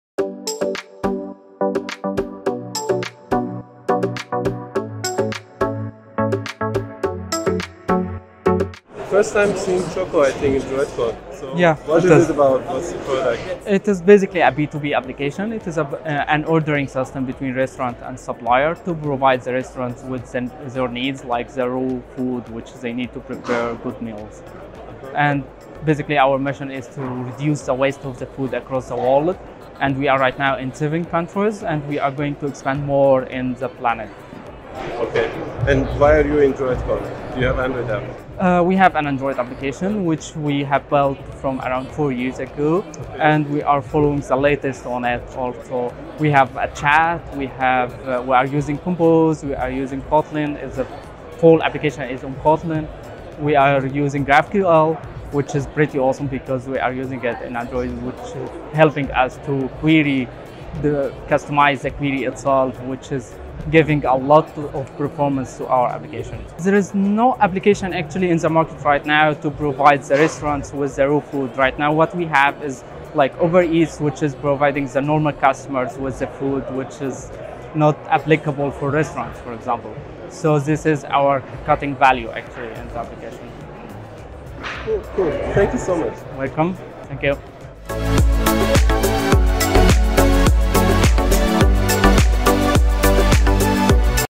Choco: save time, save money, save food — droidcon Berlin 2022
droidcon Berlin is the premier Android development conference.

Choco is one of the sponsors of droidcon Berlin 2022 and in this video, I had an interview with them about their app and the technologies used in it.